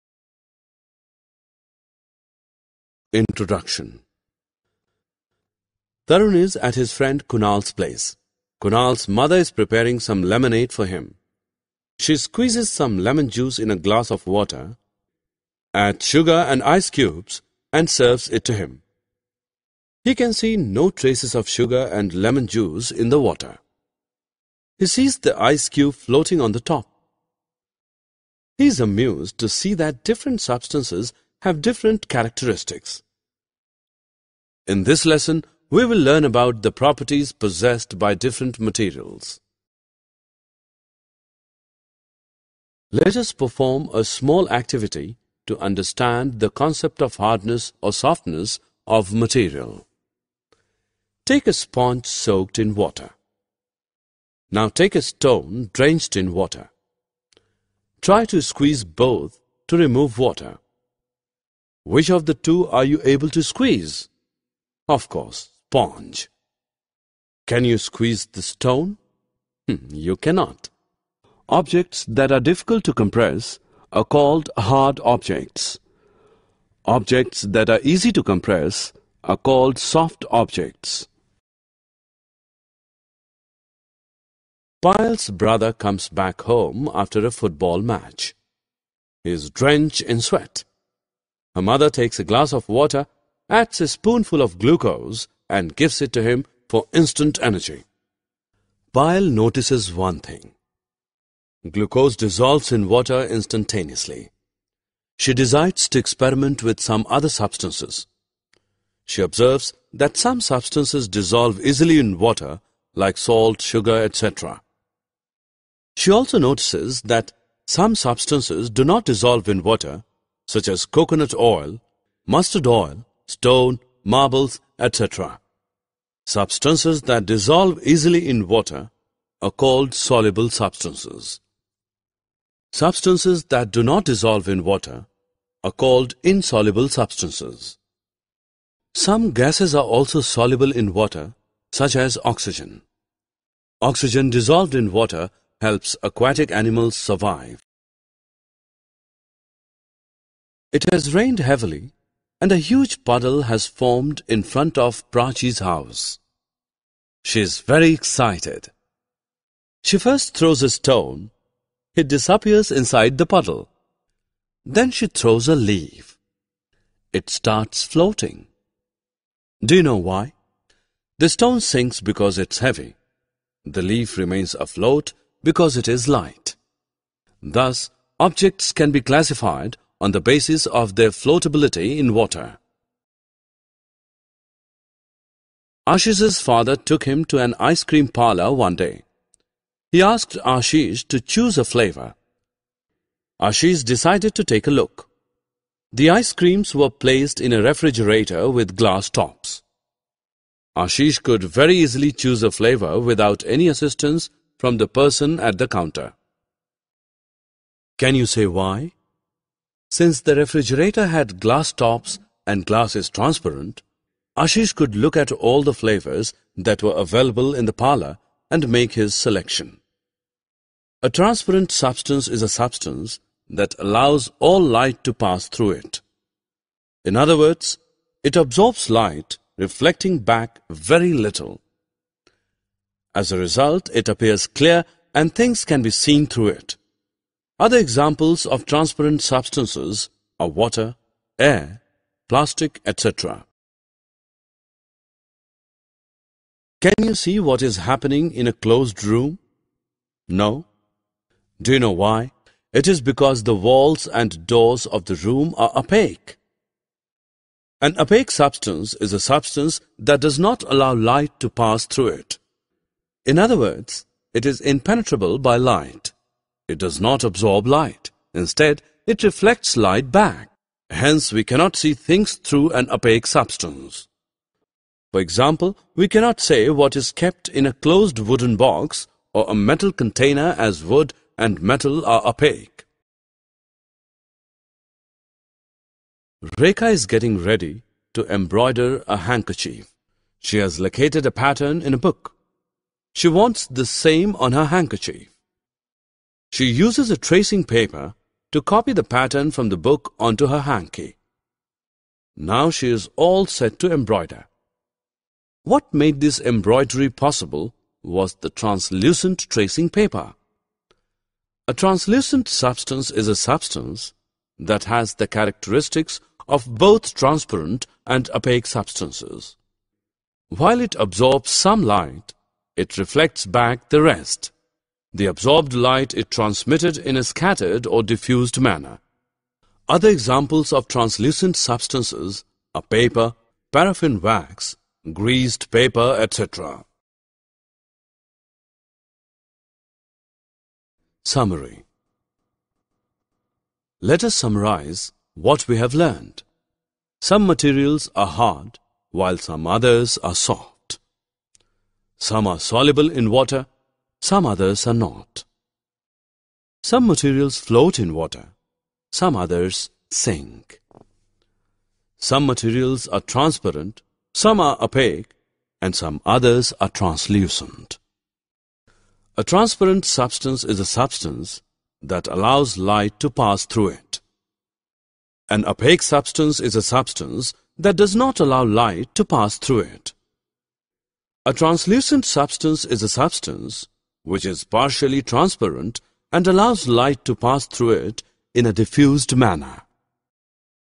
Unlocking the Secrets: Exploring the Properties of Matter! Annai Academy
Dive into the fascinating world of Properties of Matter with Annai Academy Online School! In this engaging video, we'll explore the different states of matter, their unique properties, and how they interact with each other. From solids and liquids to gases, learn how matter behaves and changes around us. Perfect for Class 6 CBSE students, this video will make science fun and easy to understand! Don't miss out on this exciting journey into the science of matter!